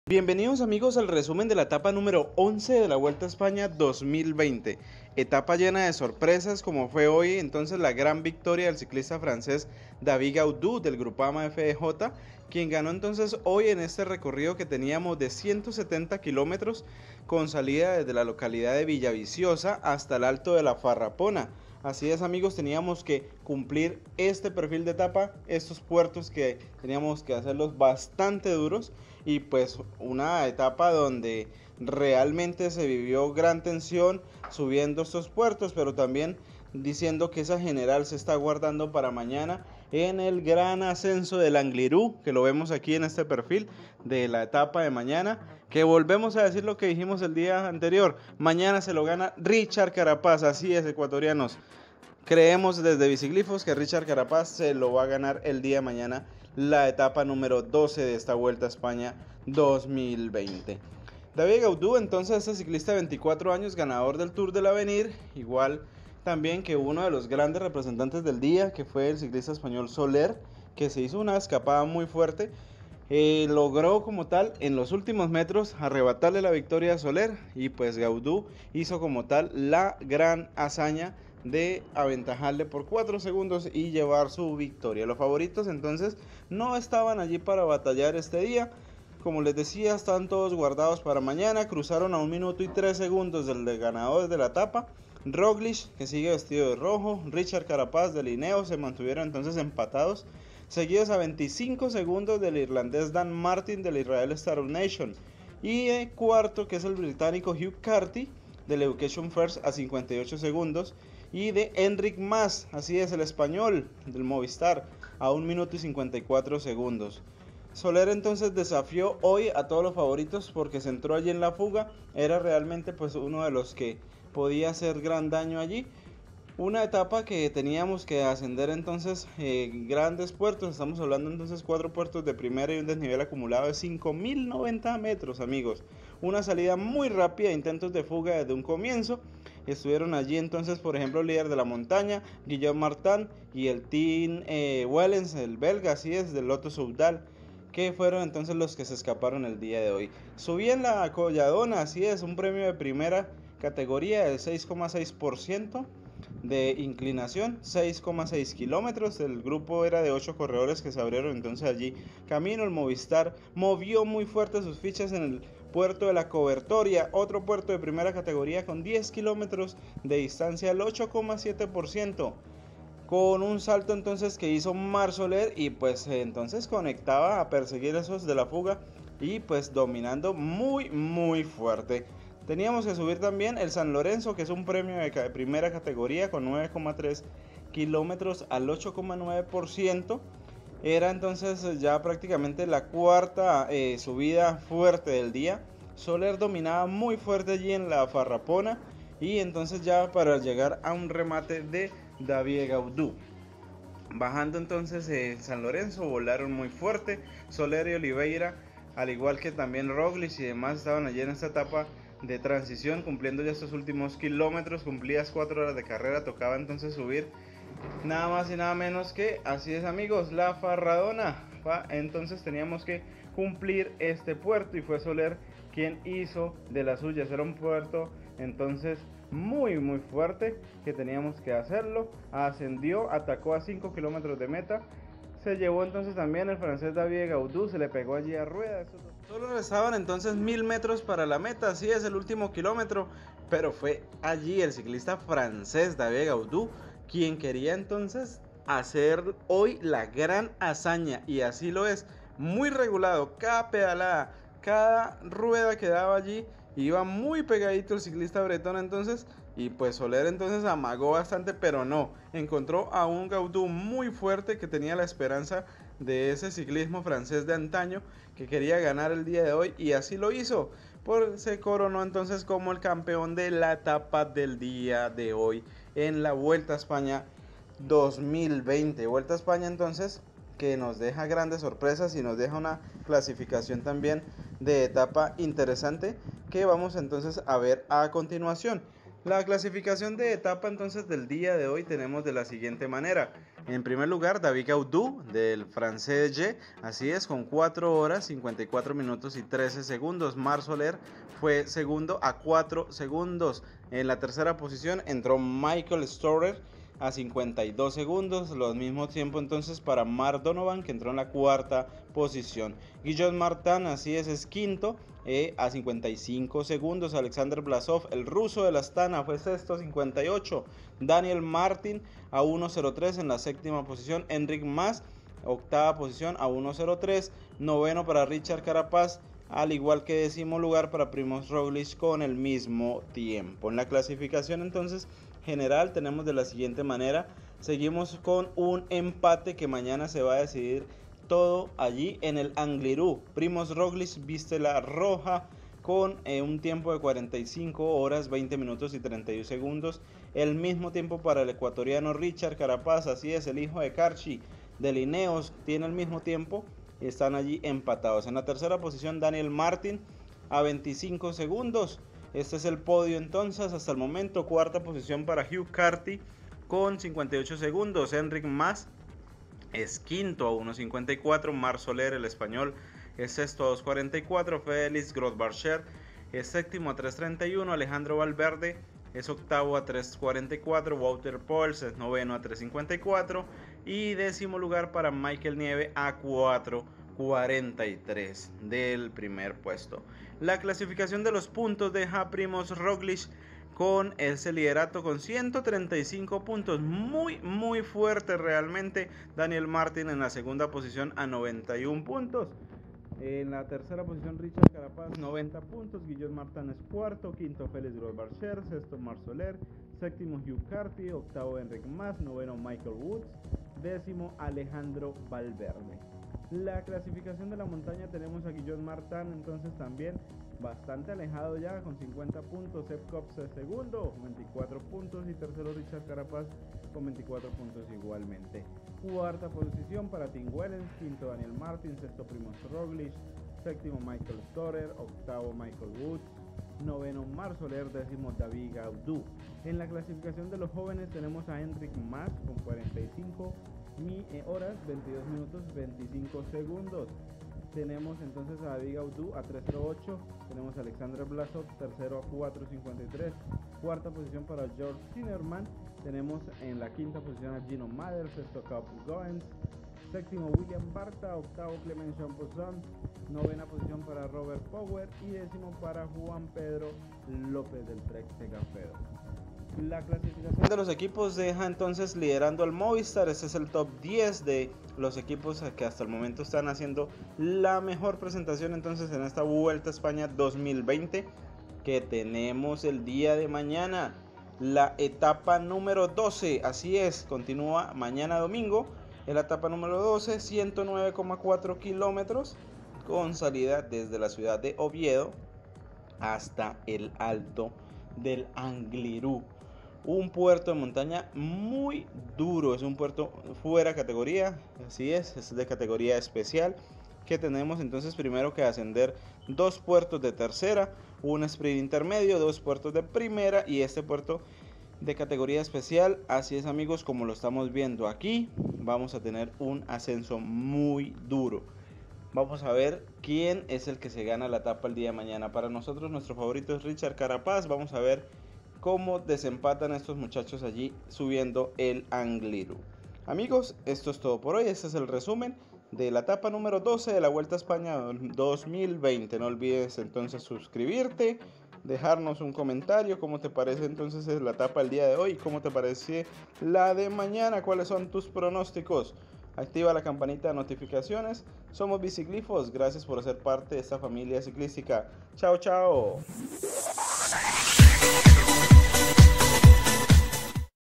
RESUMEN ETAPA 11 VUELTA A ESPAÑA 🇪🇦 2020 - EL ANGLIRU Definirá Todo
El corredor del Groupama - FDJ David Gaudu se mostró mucho más sólido en los metros finales rumbo al Alto de La Farrapona y terminó llevándose la victoria de la etapa 11 de la Vuelta a España, la cual se corrió entre Villaviciosa y Alto de la Farrapona en un recorrido de 170 kilómetros. El ciclista francés le ganó la batalla final al ciclista del Movistar Marc Soler, quien no pudo aguantar el ataque final de su rival, pues terminó pagando el esfuerzo realizado, cuando decidió despegarse del pelotón e ir a la caza de la fuga a falta de 80 kilómetros de la meta.

Mañana se viene el gran puerto del Angliru, no te pierdas el resúmen de la etapa 12